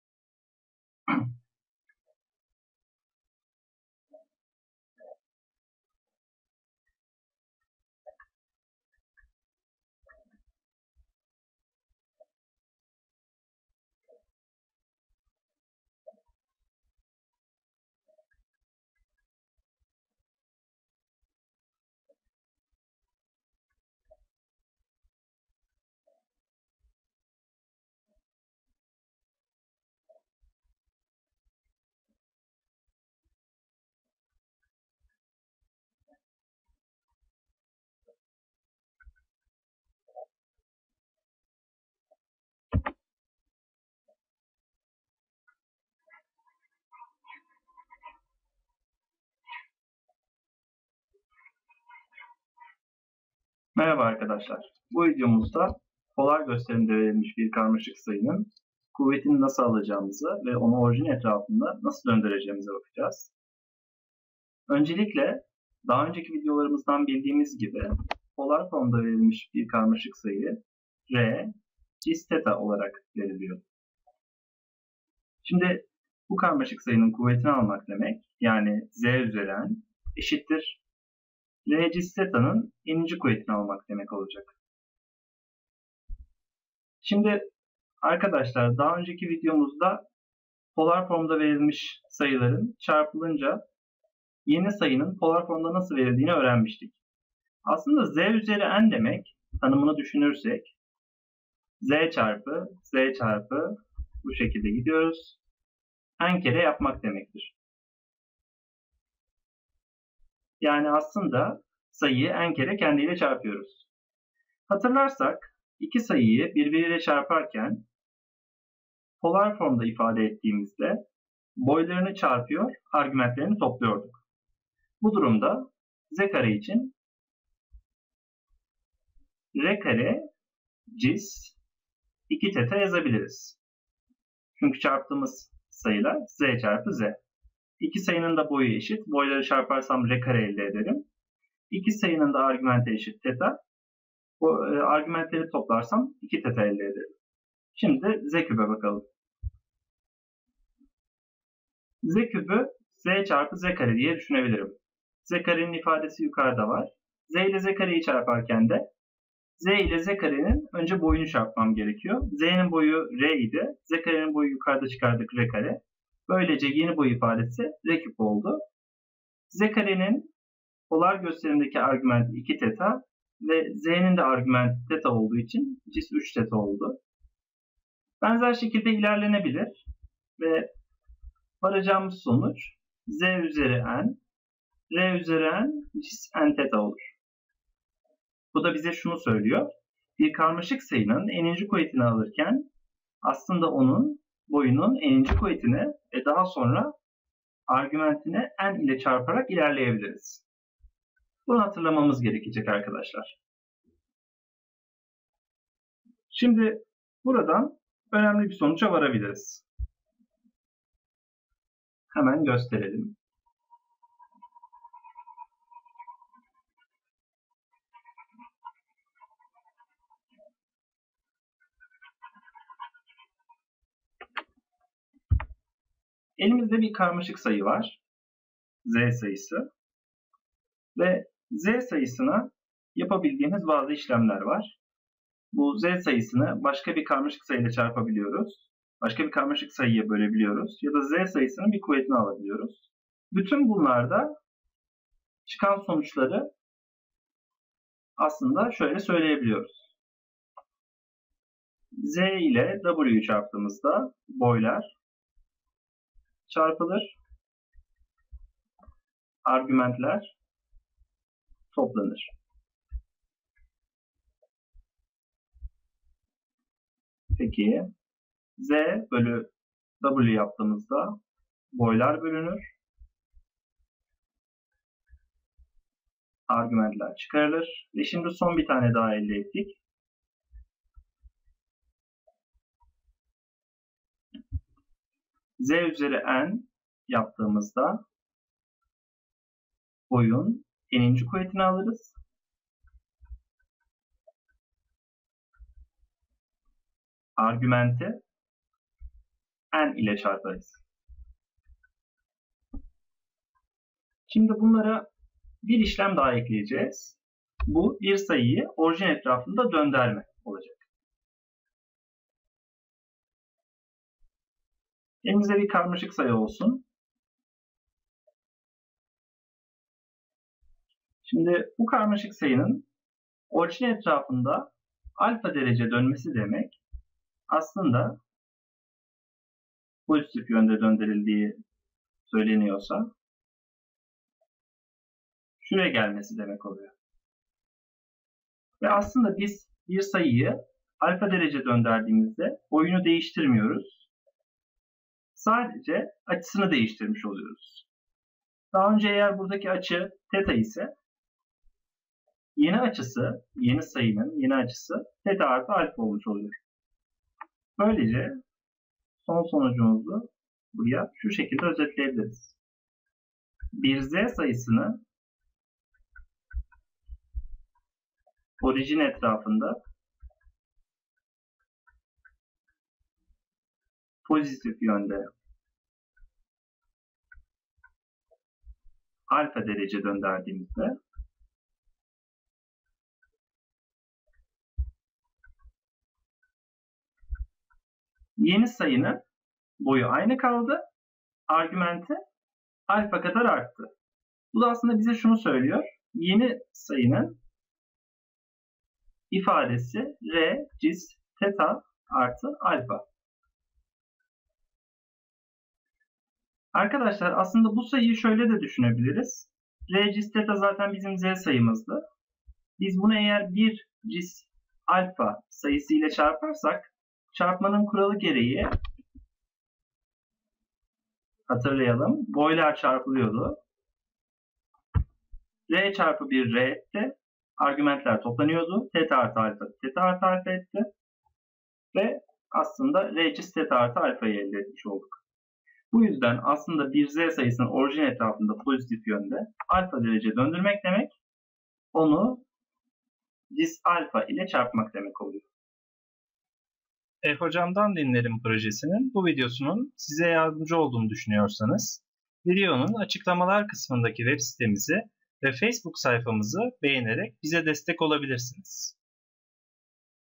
Karmaşık Sayılar -8-Polar gösterimde kuvvet alma ve orijin etrafında döndürme e-Hocam'dan dinlerim:)
Bu videomuzda polar gösterimde verilmiş bir karmaşık sayının kuvvetini nasıl alacağımızı ve orijin etrafında nasıl döndüreceğimizi inceliyoruz.